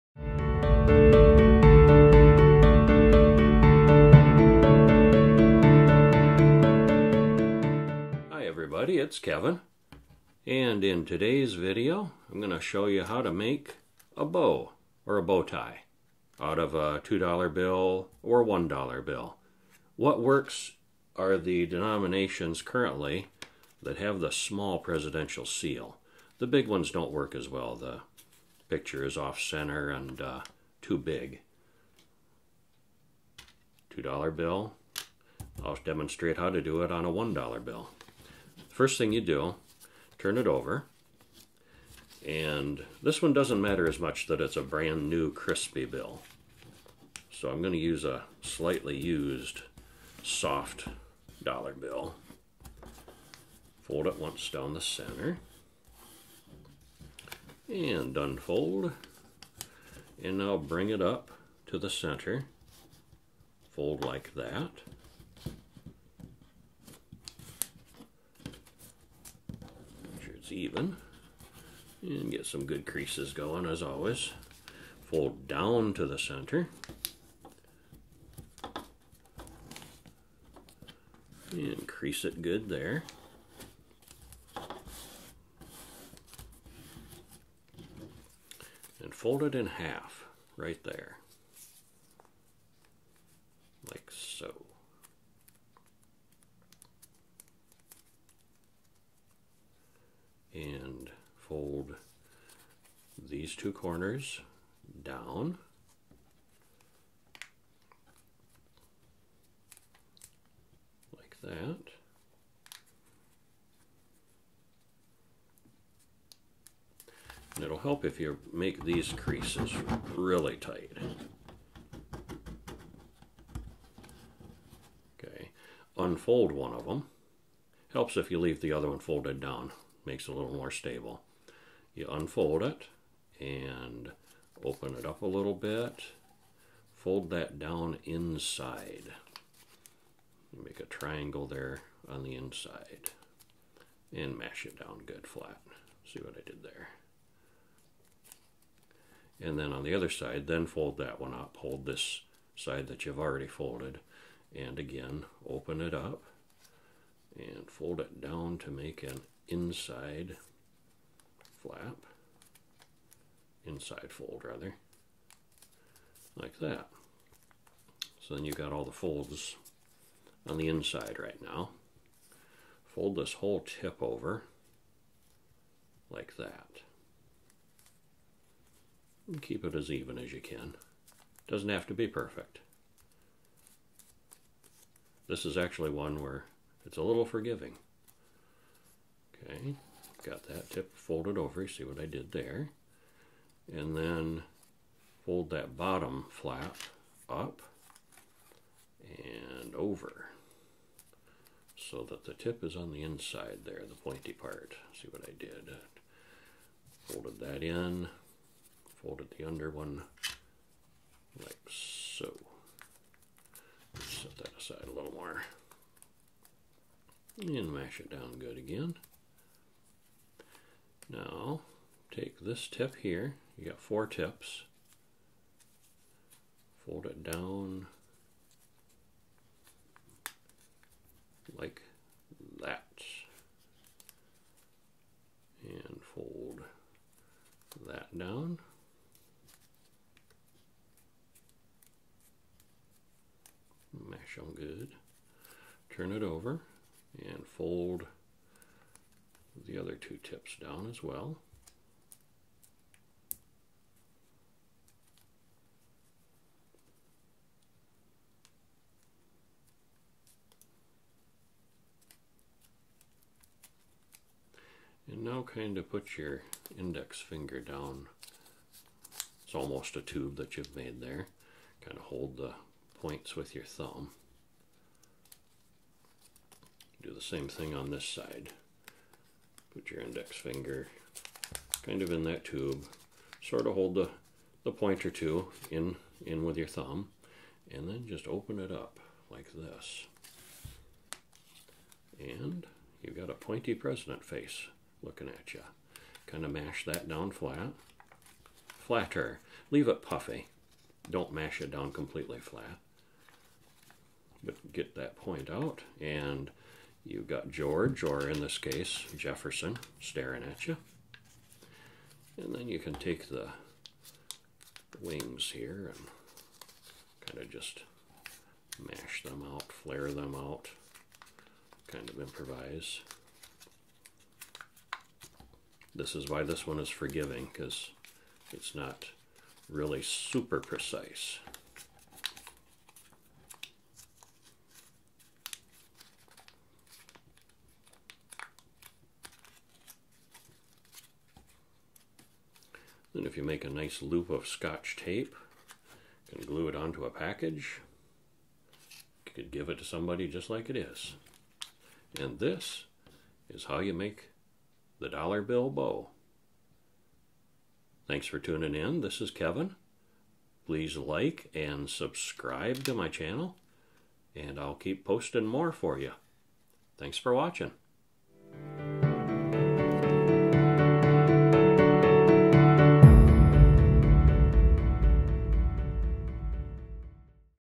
Dollar Origami: How To Fold A Bow From A Dollar (Or Is It A Bow Tie?)
In this clear step-by-step video, I'll show you how to fold an origami bow/bow tie from a one-dollar or two-dollar bill.